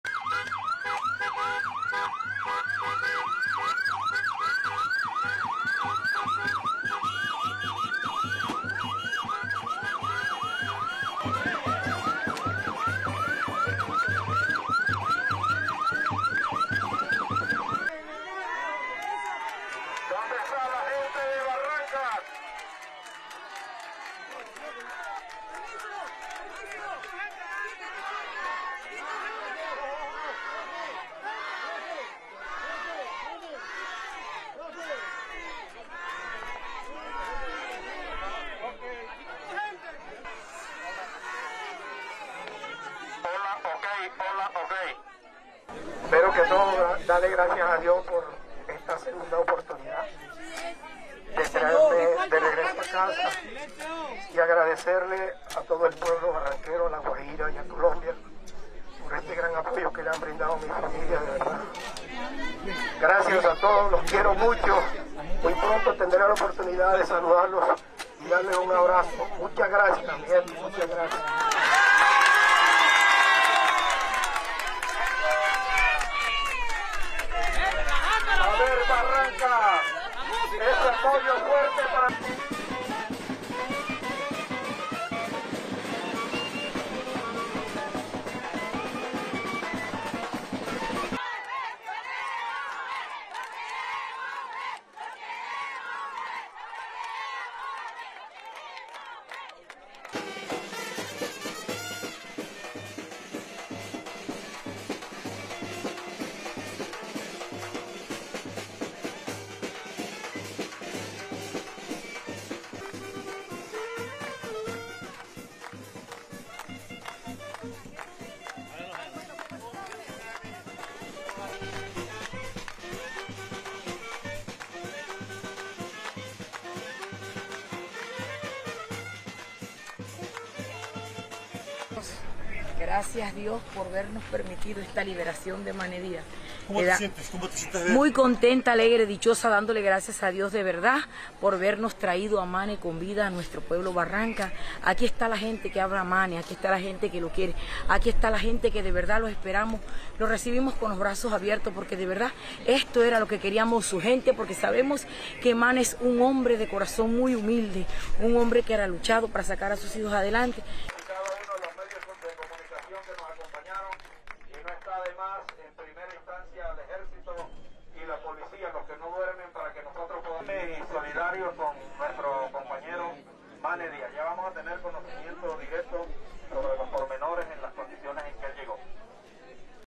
Father of soccer star Luis Diaz arrives at hometown of Barrancas, Colombia after kidnapping release
RESTRICTION SUMMARY: AP AND SNTV CLIENTS MAY USE

ASSOCIATED PRESS. - AP AND SNTV CLIENTS MAY USE
Barrancas, Colombia - 9 November 2023
1. Various of truck carrying Luis Manuel Díaz, father of Liverpool and Colombia national striker Luis Díaz, arriving to his hometown after being kidnapped by ELN guerrillas
2. Various of Diaz walking out of family home, waving
3. SOUNDBITE (Spanish) Luis Manuel Díaz, father of Liverpool and Colombia national striker Luis Díaz:
"First of all I want to thank God for having given me this second chance and for being back home. I want to thank all the people of Barrancas, the families, and Colombia for the great support they have given my family, really. Thank you all. I love you very much. Very soon, I will have the opportunity to hug you all. Thank you very much, my people, thank you very much."
4. Band playing outside Diaz family home
5. Man dancing and holding a banner reading (Spanish) "With God everything is possible"
6. Various of residents cheering
7. SOUNDBITE (Spanish) Mayulis Lindo Redondo, Barrancas resident:
"I am very happy, joyful, and blessed. I truly thank God for having brought Mane (Luis Manuel Díaz) alive to our town of Barrancas. Here are the people who love him, and we wait for him and receive him with open arms. We wanted this because we know that Mane has a humble heart who has fought to bring his children forward."
8. Various of police officers outside family home
STORYLINE:
The father of Liverpool striker Luis Díaz was released Thursday by members of a guerrilla group who kidnapped him in northern Colombia, ending a 12-day ordeal for the family.

Diaz arrived at the family home in Barrancas on Thursday afternoon, where he thanked his neighbors for the support given to his family during these days.

"First of all I want to thank God for having given me this second chance and for being back home. I want to thank all the people of Barrancas, the families, and Colombia for the great support they have given my family, really," Díaz said through a loudspeaker.

Luis Manuel Díaz’s kidnapping on Oct. 28 in the small town of Barrancas had quickly drawn international attention and appeals for his release.

On Sunday, the younger Diaz pleaded for his father ’s freedom after scoring for Liverpool in a Premier League soccer match, revealing a T-shirt saying “Freedom for Papa” in Spanish.

The release by members of the National Liberation Army, or ELN — which has called the kidnapping a mistake — was announced by the government delegation that currently is in peace negotiations with the guerrilla group.

A photo later released by Colombia's Catholic Church, which helped facilitate the release, showed the elder Díaz wearing a Chicago Bulls baseball cap, a gray shirt and a jacket.

“These really have been quite the agonizing days, with great uncertainty," Alfonso Diaz, a cousin of Luis Manuel Diaz, told The Associated Press. "But always with faith placed in God for the best outcome because our family does not deserve an attack of this magnitude.”

It was initially unclear who carried out the abduction.

But Colombia’s government announced last week that it had information that Díaz was kidnapped by an ELN unit.

The ELN later acknowledged the kidnapping, saying it was a mistake and that the group’s top leadership had ordered the elder Díaz’s release.

Diaz’s father was received Thursday by representatives of the United Nations and the Catholic Church in the area of the mountain range Serranía del Perijá, where he received primary medical care.

AP Video shot by Marko Alvarez